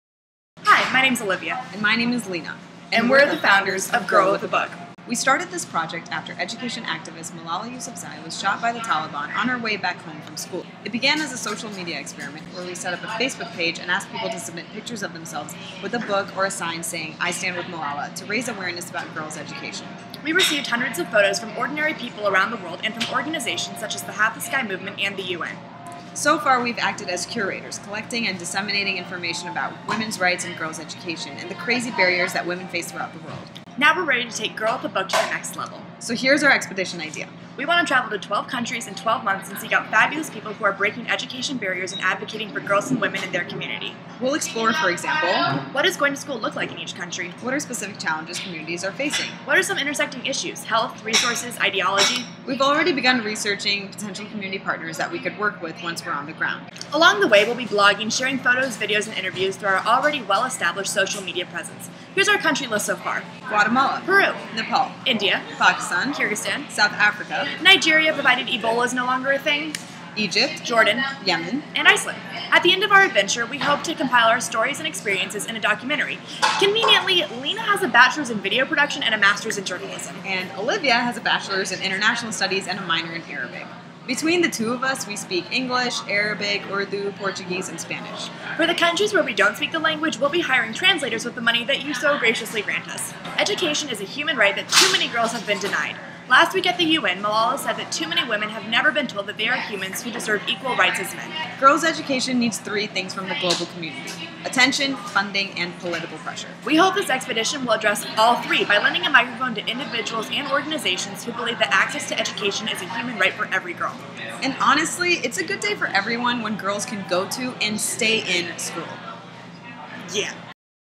2014 Expedition Granted Finalist: Girl With a Book | National Geographic
12 countries, 12 months, and 2 women take
GIRLWITHABOOK abroad & document great folks breaking barriers for girls & education.

Vote for Olivia Curl and Lena Shareef's project at ExpeditionGranted.com. One winner will be selected by your votes to receive $50,000 to fund their dream expedition!